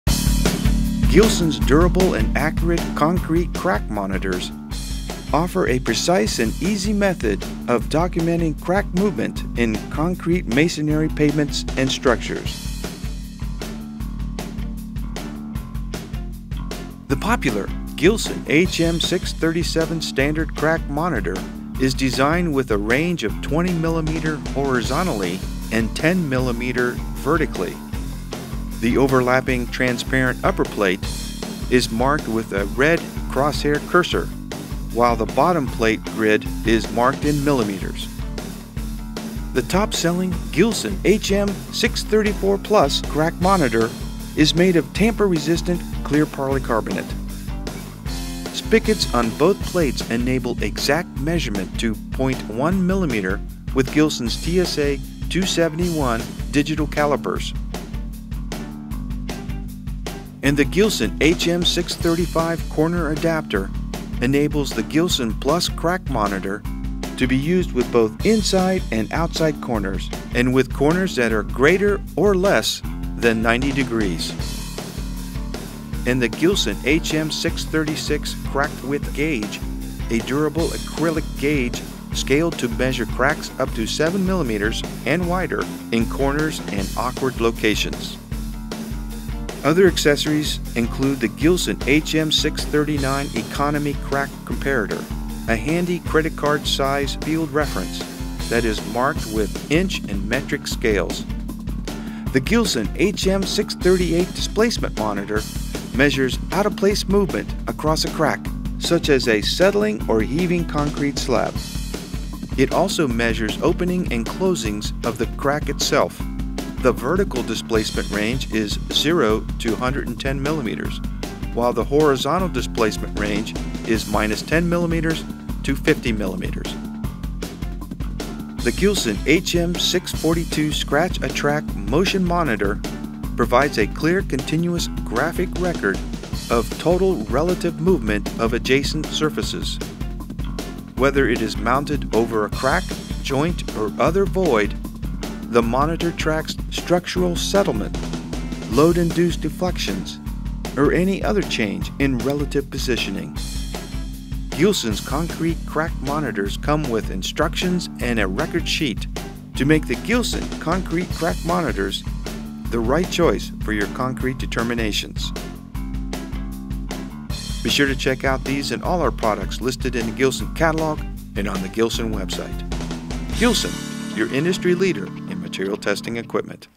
Gilson Concrete Crack Monitors
HM-637 Standard Crack Monitor is a simpler design with a range of 20mm horizontally and 10mm vertically. The overlapping upper plate is transparent and marked with a red crosshair cursor. The white bottom plate measuring grid is marked in millimeters. There are no spigots for use with calipers. Readability is 1mm.

HM-634 Plus Crack Monitor is vandal-resistant clear polycarbonate with adjustment slots for screws ensure secure placement. After affixing, temporary pegs are removed so plates move independently. Spigots on both plates enable exact measurement to 0.1mm with calipers. (Gilson recommends TSA-271 Digital Caliper.) A second Crack Monitor may be positioned for monitoring in three dimensions.

HM-635 Corner Adapter enables Crack Monitors to be used in corners with greater or less than 90˚ angles.

HM-636 Crack Width Gauge assists those who survey and report on damaged buildings. This durable acrylic Gauge is scaled to measure cracks up to 7mm and wider in corners and awkward locations.

Economy Crack Comparator is a handy, credit card-sized field reference that slips easily into your pocket or wallet. Clear plastic card is marked with inch and metric scales as well as a range of common crack sizes.

HM-638 Displacement Monitor measures out-of-place movement across a crack, such as settling of a floor slab. It also measures opening and closing of the crack itself. Displacement range is 110mm. Horizontal range is -10—+50mm.

The Scratch-A-Track Motion Monitor provides a clear, continuous graphic record of total relative movement of adjacent surfaces. Whether it is mounted over a crack, joint or other void, the Monitor tracks thermal movement, structural settlement, load-induced deflections, or any other change in relative positioning. The unit is small, lightweight, inexpensive and weather resistant.

The plastic single-use Recording Pad has a target area coated with a white film and convenient blanks to record written data such as date, location, etc. The Pad is bonded to one surface using super-glue or epoxy (not included). The nickel coated spring steel Finger assembly is then bonded to the opposing surface and adjusted with a thumb screw so that the stainless steel Scriber moves over the surface of the Pad, it leaves a thin, well-defined black scratch to permanently record its movement. Finger and Scriber assembly may be used over and over.